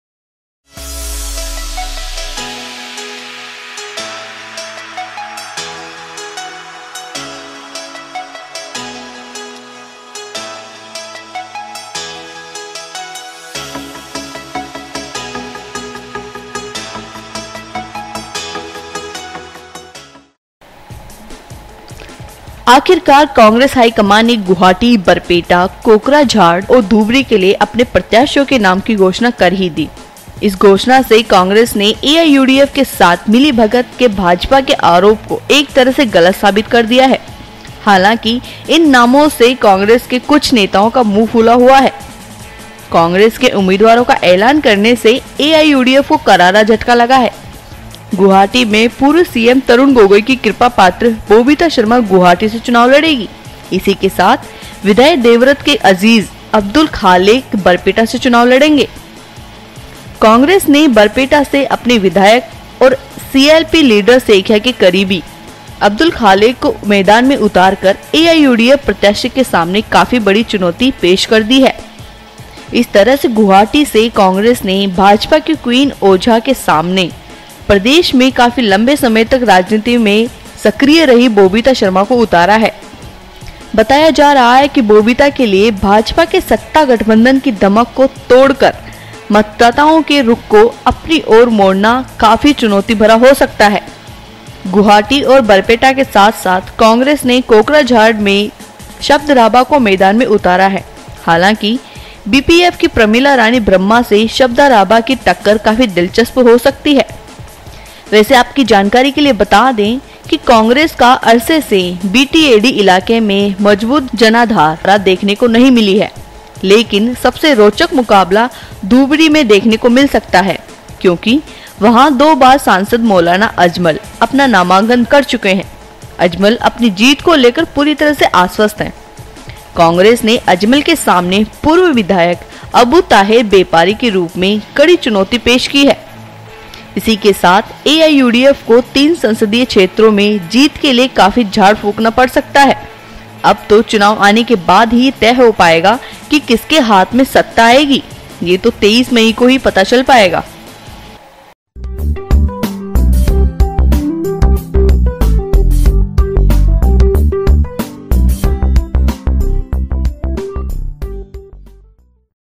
गुवाहाटी से बोबीता और बरपेटा से खालेक देंगे विपक्षी पार्टी को कांटे की टक्कर
कांग्रेस के उम्मीदवारों का ऐलान करने से एआईयूडीएफ को करारा झटका लगा है। गुवाहाटी में पूर्व सीएम तरुण गोगोई की कृपापात्र बोबीता शर्मा चुनाव लड़ेंगी और बरपेटा से विधायक देवब्रत सैकिया के अजीज विधायक अब्दुल खालेक चुनाव लड़ेंगे।